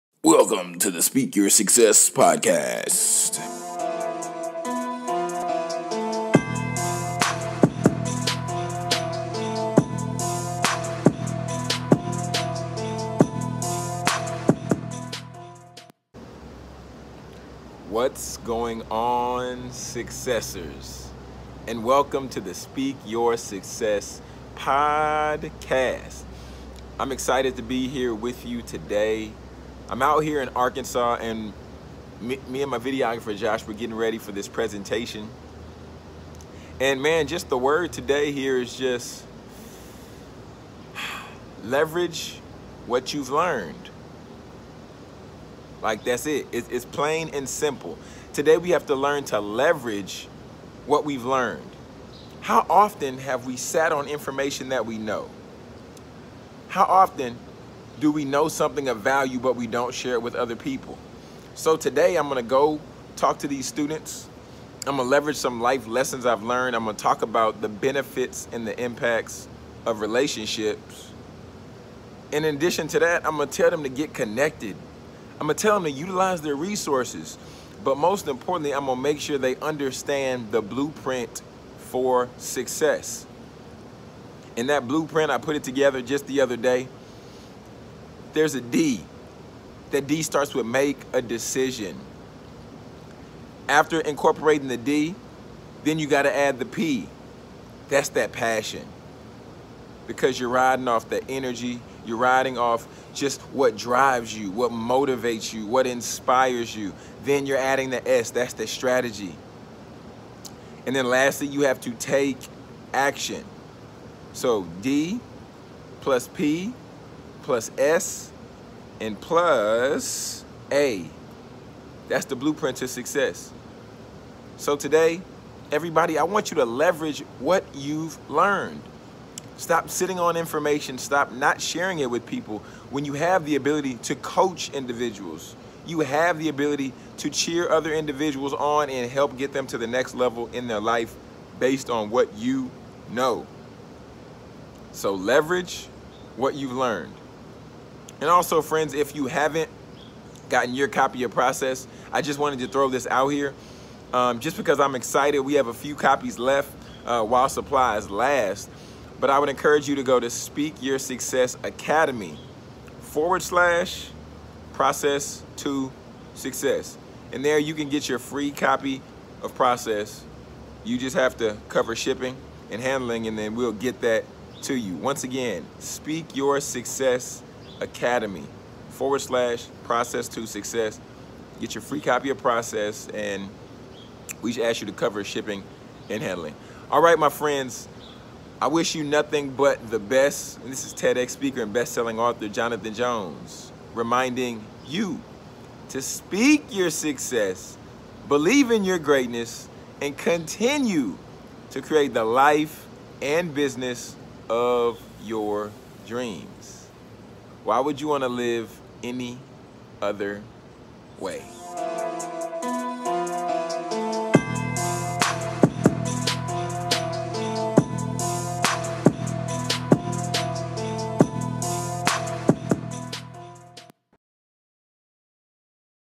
Leveraging What You've Learned FastbreakFriday x
How often do we not use what we have learned because we feel that we aren't adequate or don't have enough experience to share with others?